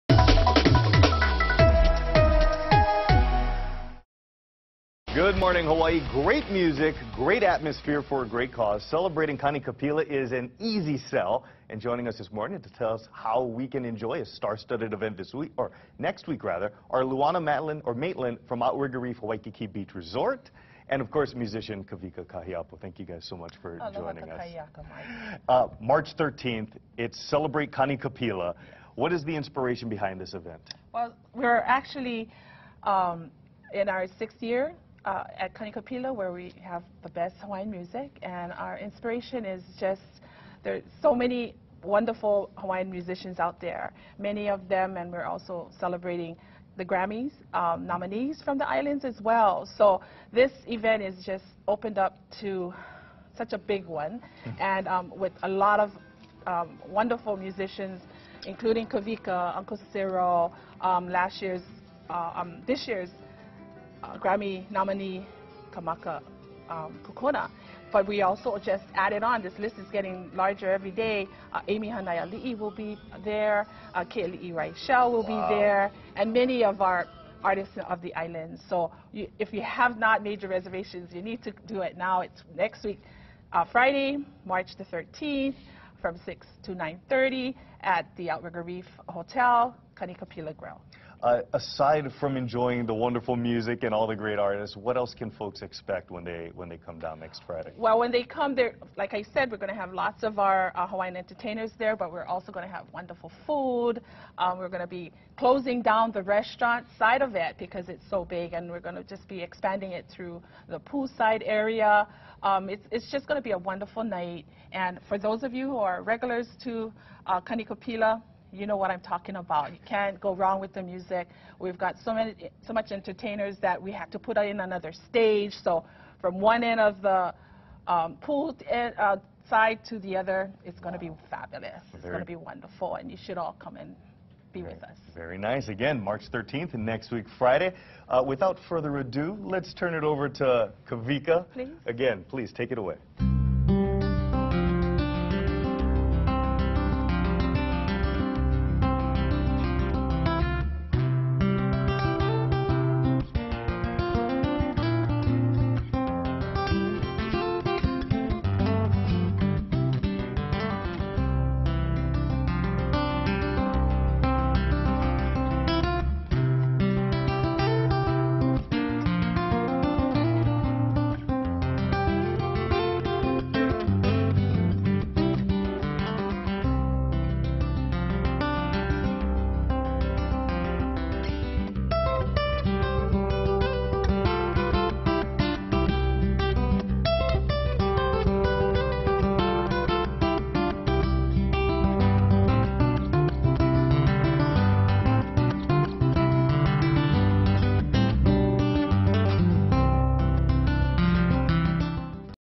Kani Ka Pila in Waikiki
Celebrate great music with some of Hawaii's finest musicians at the Kani Ka Pila celebration at the Outrigger Reef next Friday.